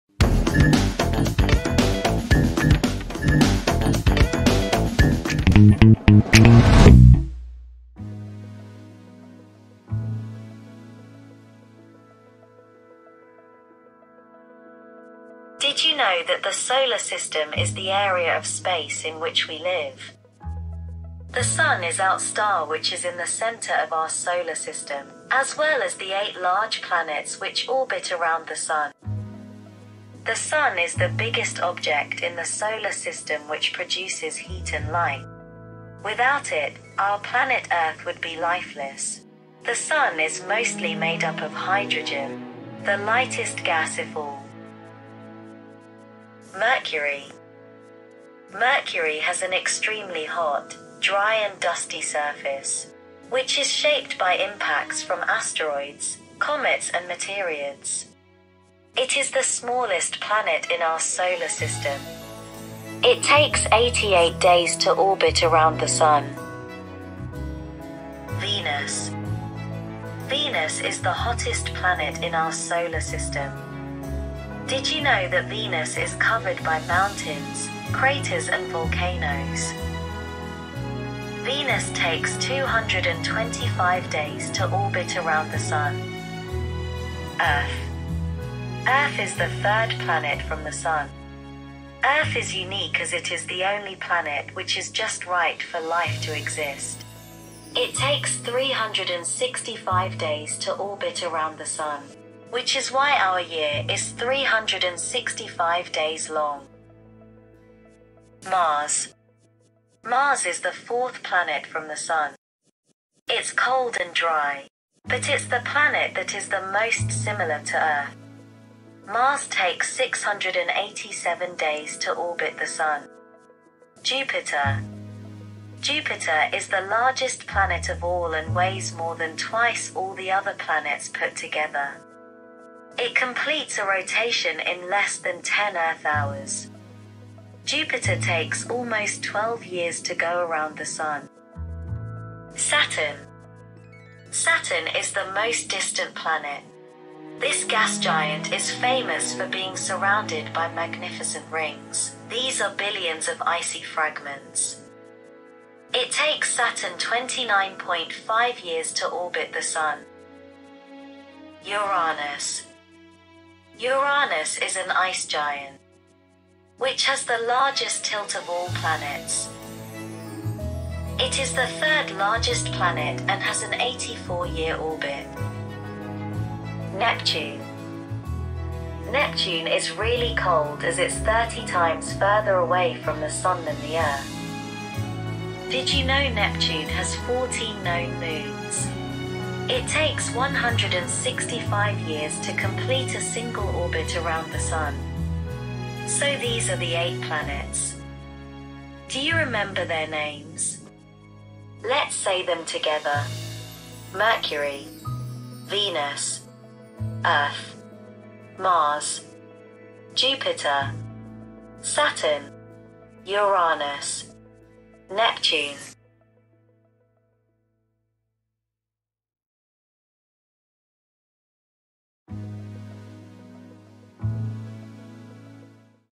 Planets in Our Solar System | Learn The 8 Planets | Kids Educational Study
Hey Kids,
Let's have a look at Our Solar System in which we live. We will learn about our Star, which is in the centre - The Sun - as well as the eight large Planets: Mercury, Venus, Earth, Mars, Jupiter, Saturn, Uranus and Neptune. Do you know how long it takes each planet to Orbit around the Sun? You will certainly find out in this video.

We will find out some interesting facts about each Planet. Lets go!